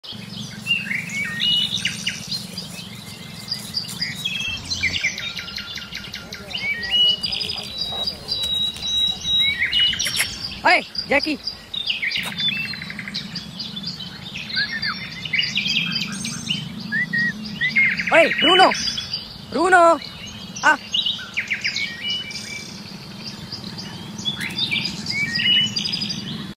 Bruno the Saint Bernard
Beautiful Saint Bernard in Pakistan 🇵🇰 ..